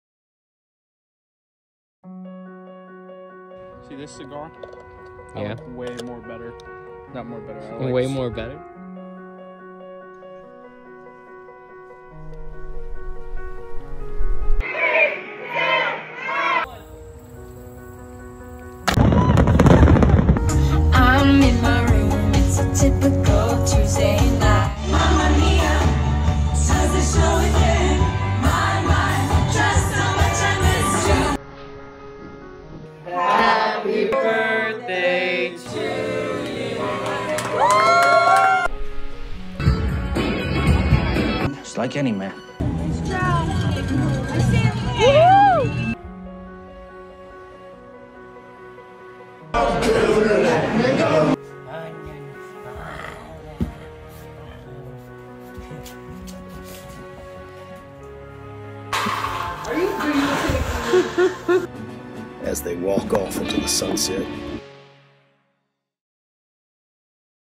Connections -Short Film
Connections is a film compiled from old footage repurposed to show the lead character's struggle with loneliness. Upon meeting new people he sees the value of connections, which causes him to have a new outlook on life.
Instagram:   / JG_Filmmaking & joseph_guzzone